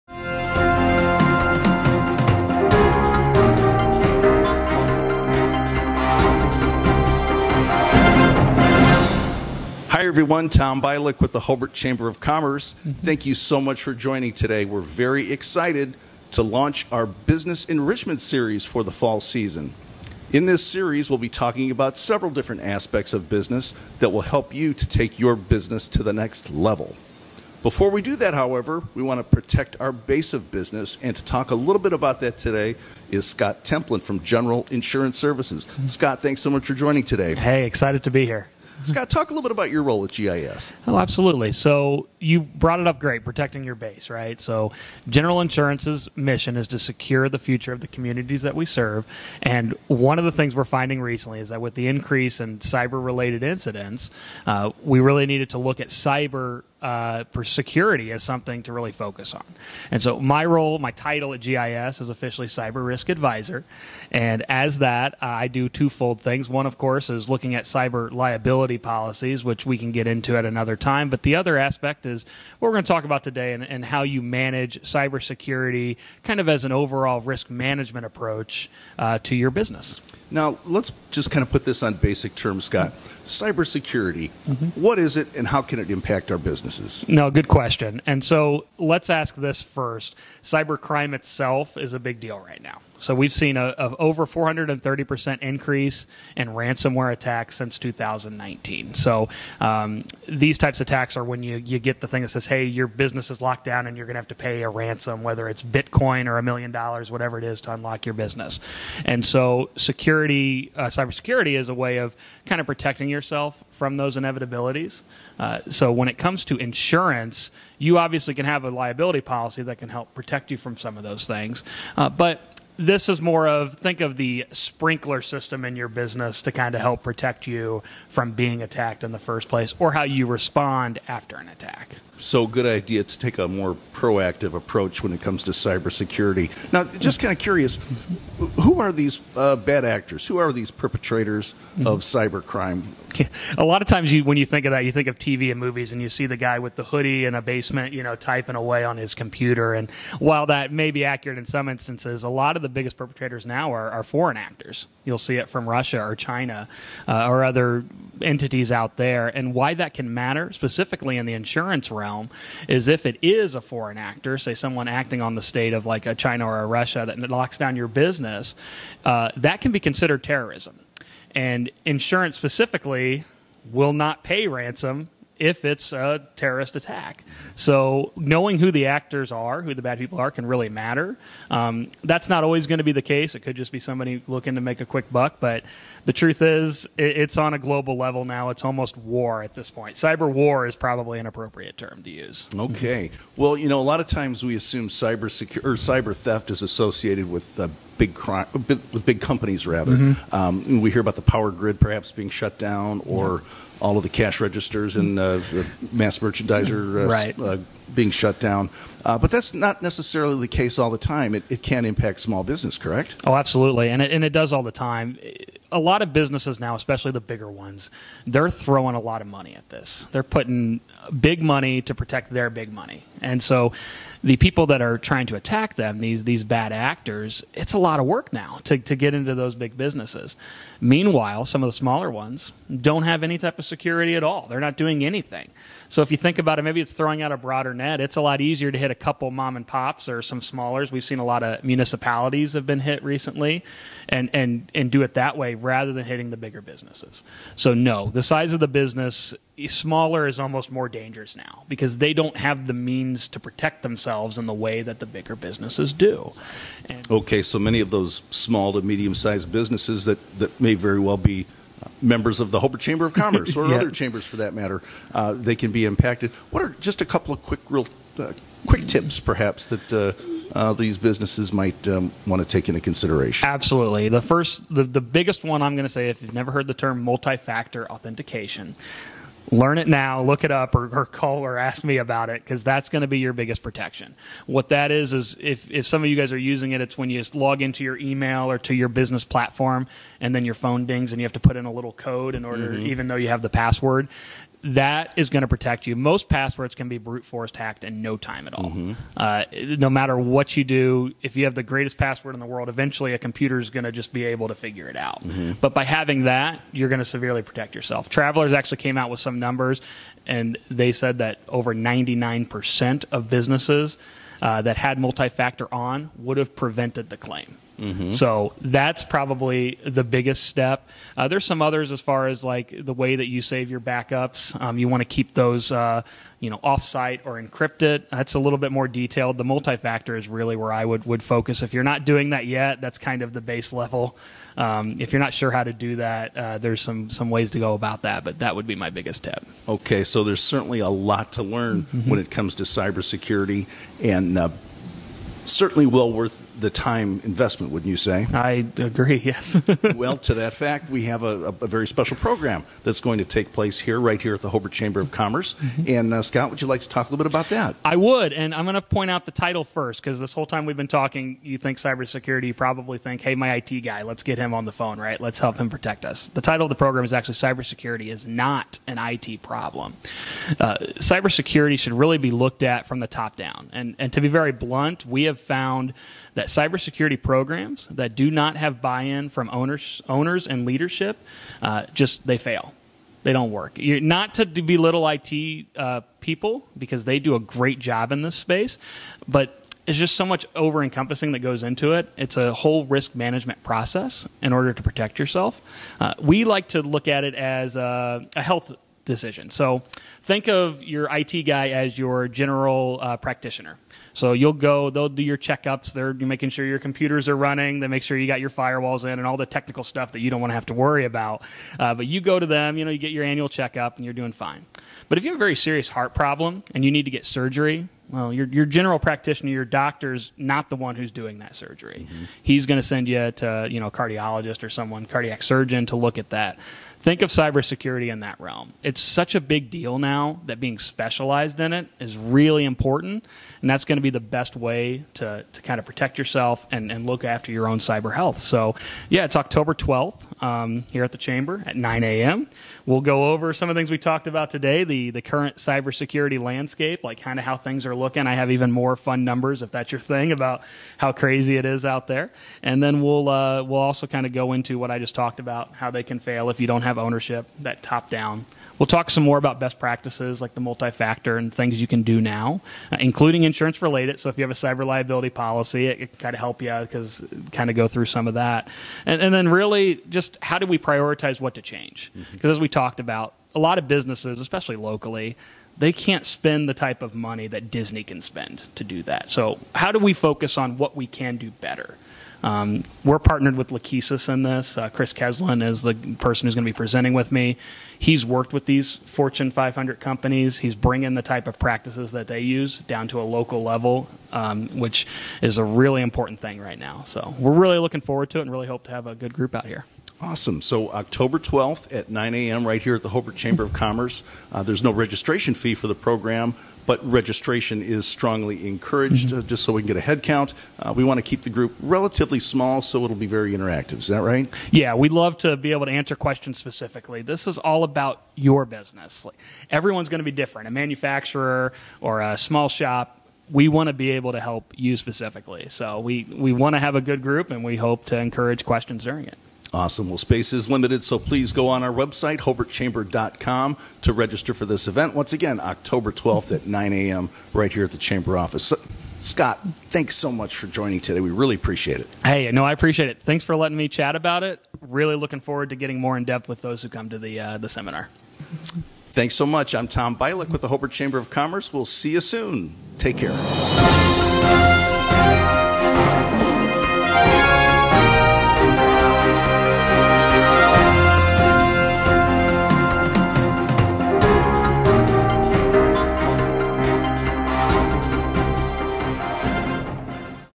Business Enrichment Series: Cyber Security Is NOT An IT Problem
How is cyber risk accounted for on your balance sheet? Do you understand the drastic effect to reputation your business could face with a cyber incident?

Cybersecurity does not start and end with your IT department. It truly is a business concern that requires collaboration between ownership, operations, relevant specialist, and IT.

Dive in with Lachesis to discover:

The current cybersecurity landscape
Cybersecurity programs fail without ownership participation
Best practices for avoiding and responding to cyber threats
How to prioritize vulnerabilities
Questions every executive should ask about their cyber profile
Case studies showcasing steps you can take right now to improve your cyber health